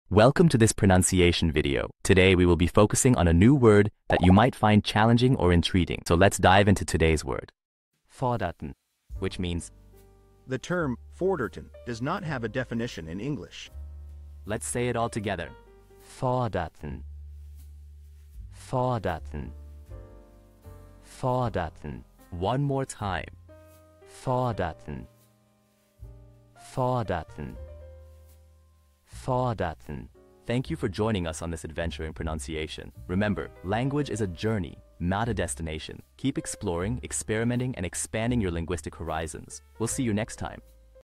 How to pronounce Forderten
Master the Pronunciation of 'Forderten Which means demanded' - which means : The Term "demanded" does not have a definition in English. 📝 with @PronunciationGerman 🎓🗣️ - [Your Guide to German]

Learn to pronounce 'Forderten' perfectly in German with our easy and engaging tutorial by @PronunciationGerman! 📖📽️ In this comprehensive video, we provide you with native speaker examples 🗣️👂 and phonetic breakdowns 🎵 to ensure you get the pronunciation just right.

How to say Demand in German